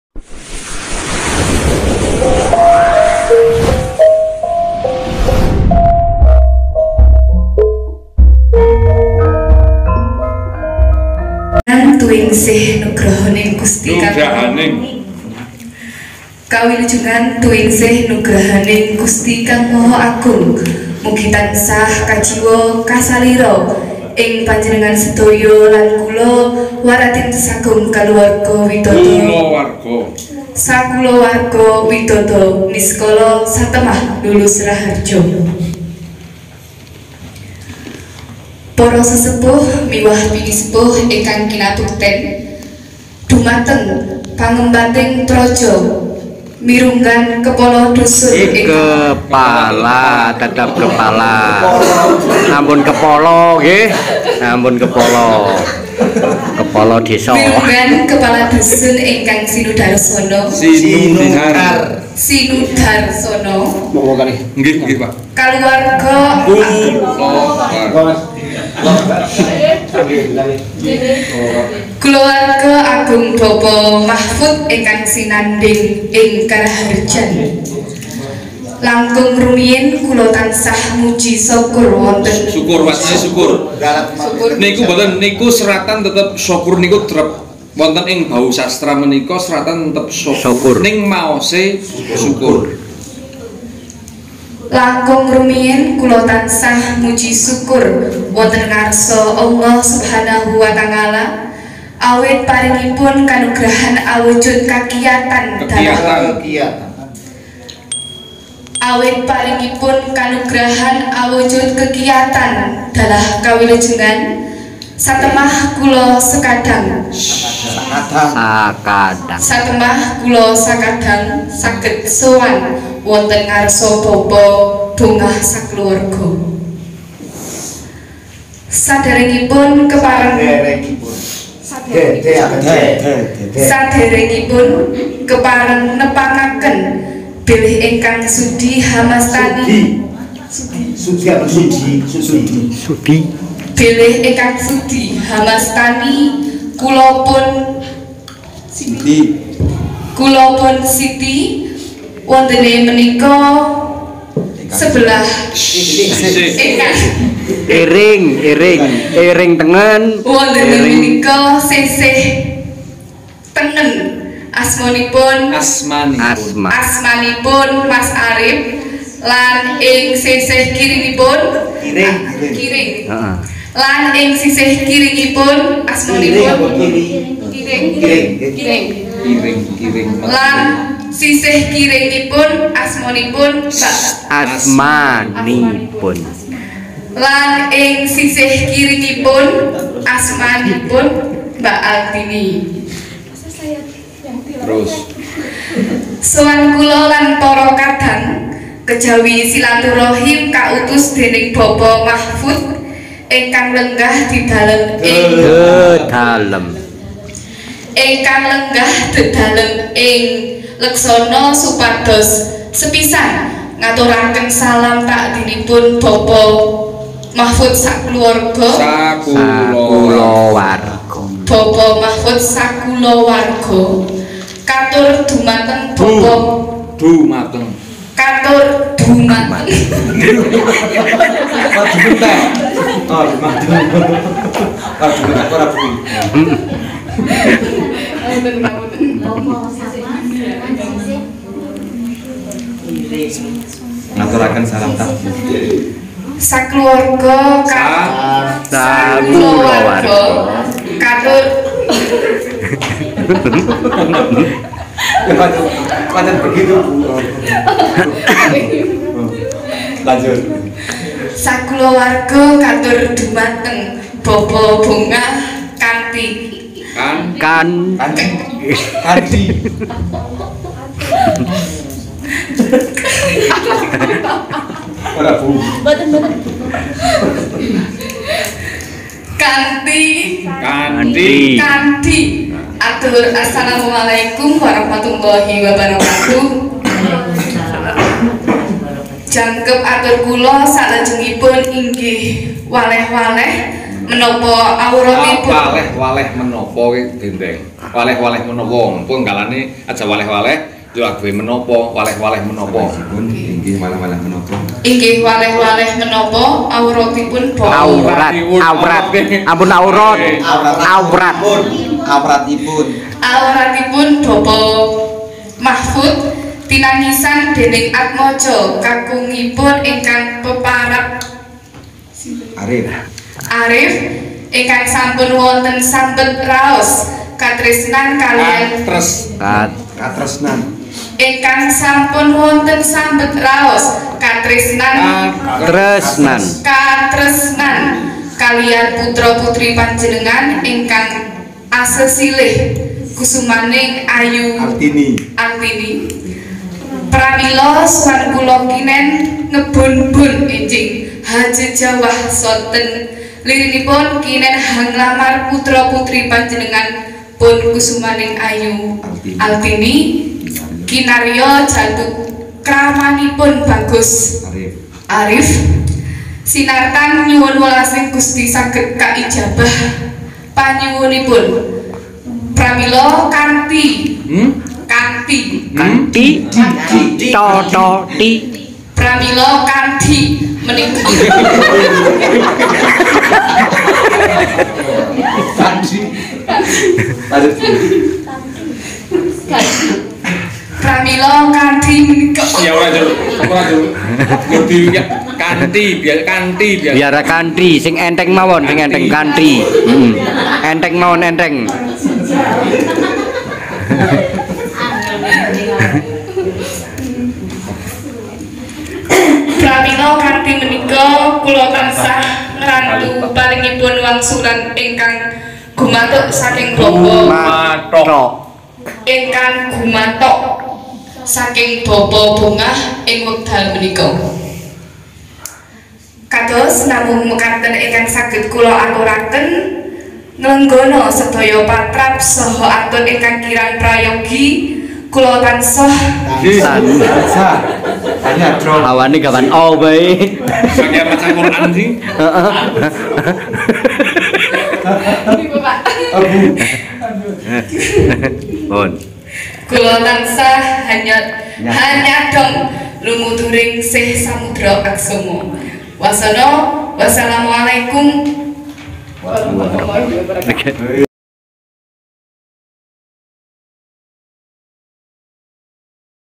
Gladhen LAMARAN Bahasa Jawa Sederhana dan Mudah
Gladhen LAMARAN Bahasa Jawa Sederhana dan Mudah sekaligus melestarikan adat dan budaya khususnya bahasa Jawa yang sudah hampir punah bahkan orang Jawa sendiri lebih memilih bahasa nasional pada kali ini saya akan menyajikan Gladhen LAMARAN Bahasa Jawa Sederhana dan Mudah